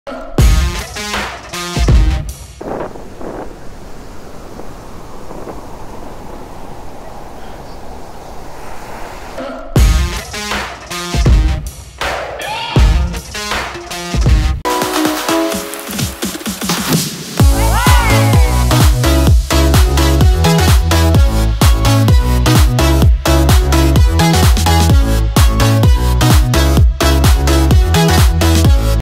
Chandeleur : fais la crêpe sur la plage !
Pour la Chandeleur, il y a faire LA crêpe sur la plage ou... faire DES crêpes sur la plage ! La team Estérel Côte d'Azur a testé les 2, toi aussi choisis ton camp !